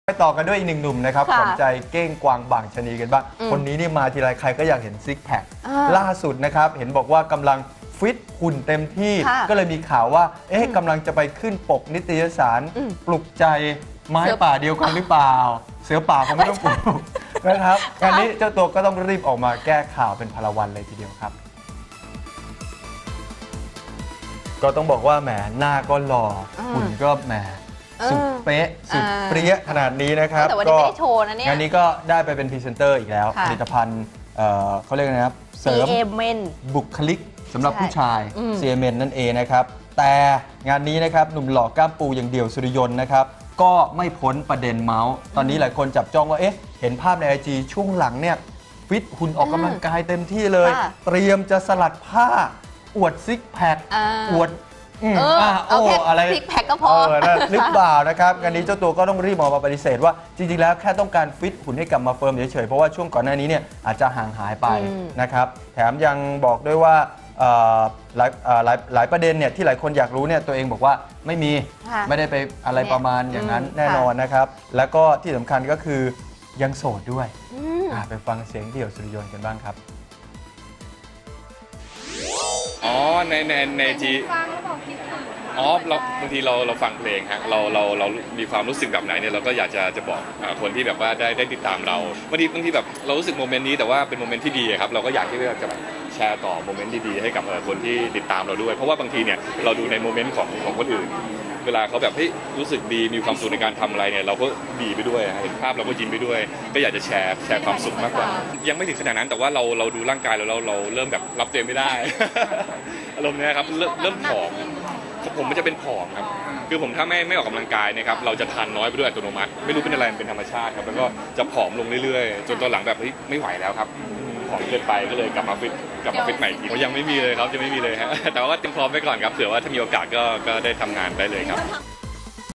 "เดี่ยว" ปัดฟิตหุ่นลงปกเซ็กซี่ แจงโพสต์ "คิดถึง" แค่อินเพลง
ติดตามรายละเอียดเพิ่มเติมได้ที่